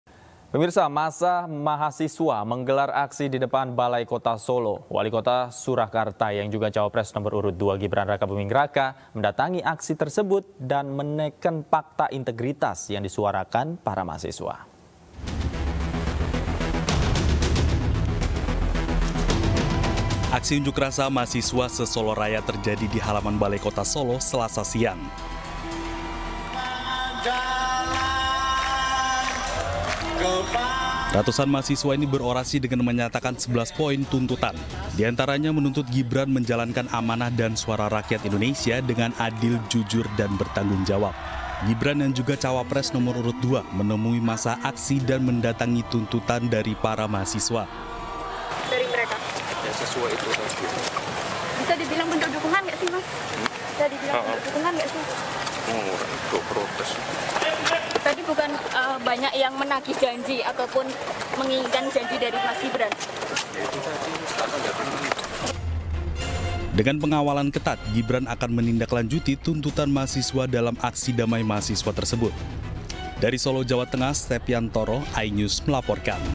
Temui Ratusan Mahasiswa Pendemo di Balkot Solo, Gibran Tanda Tangani Pakta Integritas
Wali Kota Solo Gibran Rakabuming Raka menemui ratusan mahasiswa menggelar aksi demonstrasi atau demo di depan Balai Kota (Balkot) Solo, Jawa Tengah, Selasa (6/2/2024).

Dalam aksi tersebut para mahasiswa nampak membawa sejumlah spanduk berisi tuntutan kepada Gibran yang juga merupakan salah satu calon wakil presiden (Cawapres) di Pilpres 2024.